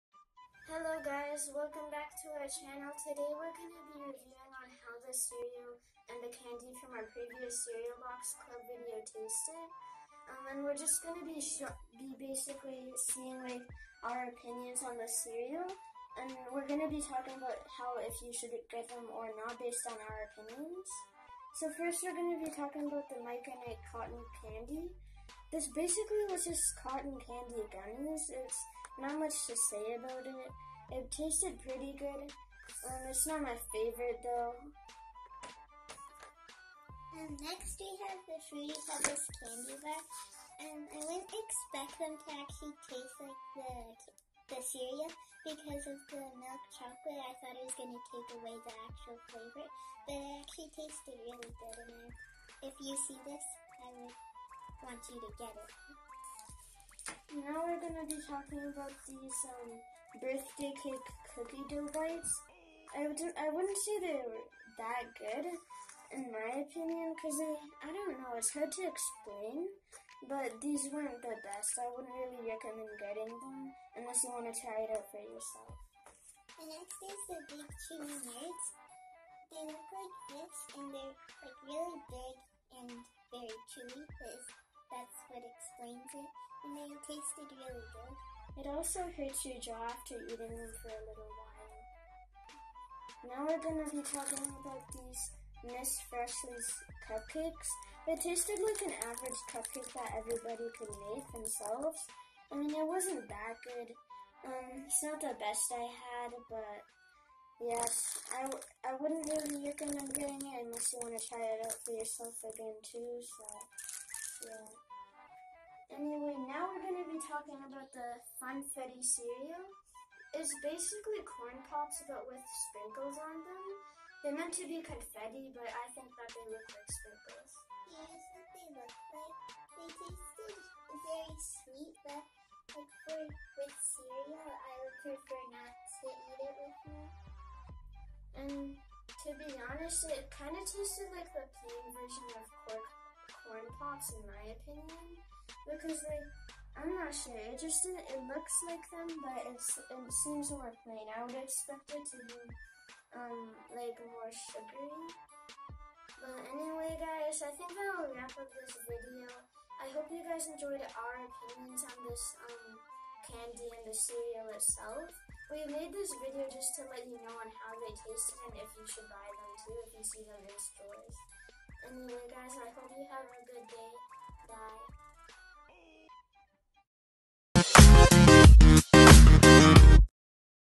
How They Taste
This video was made for you to know how the candies / cereal tasted from the cereal box club part 3 video was! I recommend watching that video before this one.